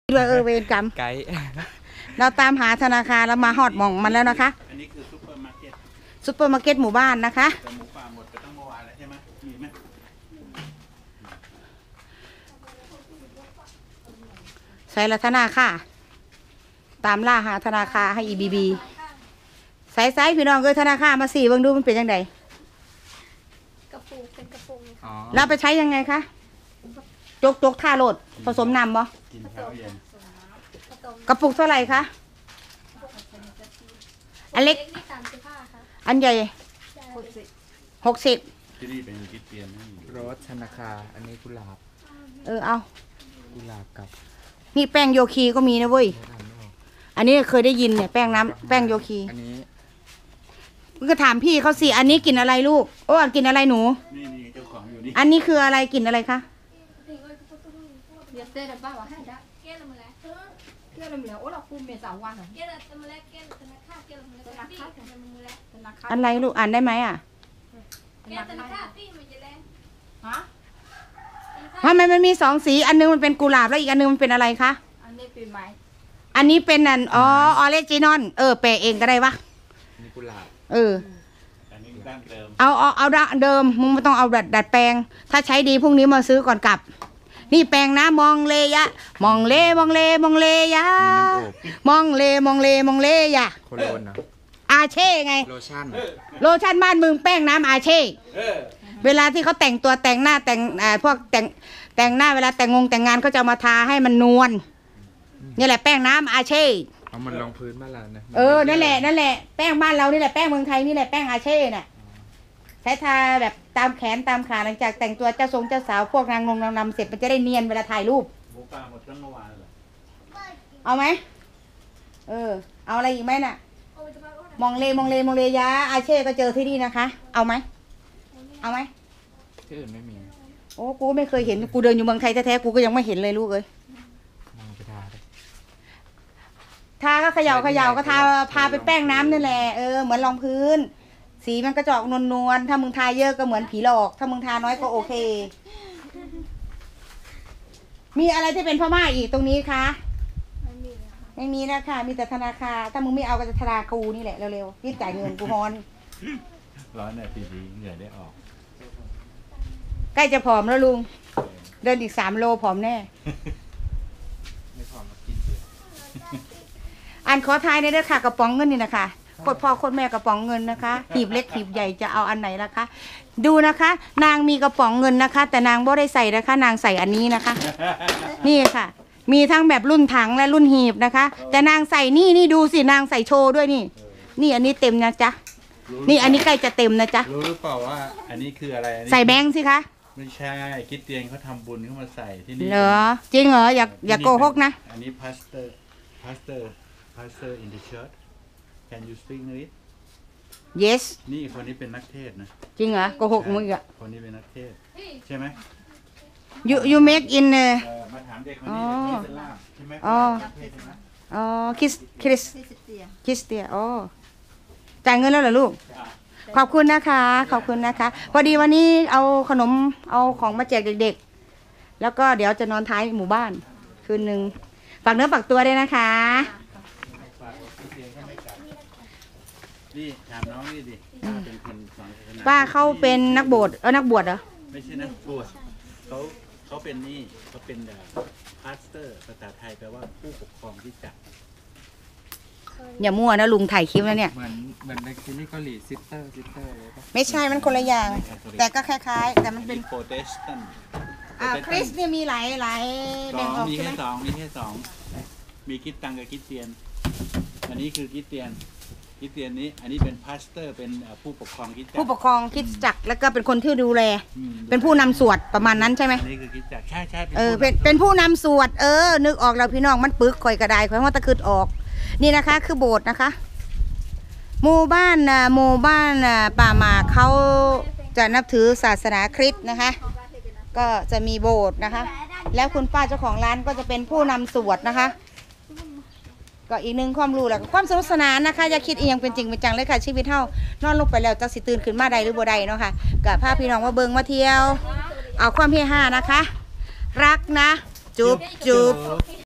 ตามล่าหาทะนาคาไห้บีบีที่ซุปเปอร์มาเก็ตบ้านป่าหมาก สามร้อยยยอด ประจวบคีรีขันธ์
ตามล่าหาทะนาคาไห้บีบีที่ซุปเปอร์มาเก็ตบ้านป่าหมาก สามร้อยยยอด ประจวบคีรีขันธ์ ที่นี่มีทั้งพุทธ และคริส แต่ทุกคนอยุ่ร่วมกันได้ เป็นหมู่บ้านในป่าที่ไม่มีไฟฟ้าใช้ แต่มีความสงบมาก ส่วนร้านค้ามีขายทุกอย่าง ไม่ว่าจะเป็นของเบ็ดเตล็ดหรือเครื่องดื่ม แต่ปกติน้ำดื่มใช้จากลำธารดังั้นจึงไม่มีน้ำดื่มขาย มีน้ำอัดลม หรือแอลกอฮอร์ขายเท่านั้นเท่าที่ถามและไปขอซื้อน้ำต่อจากชาวบ้านที่เขามีไว้ไห้พ่อดื่ม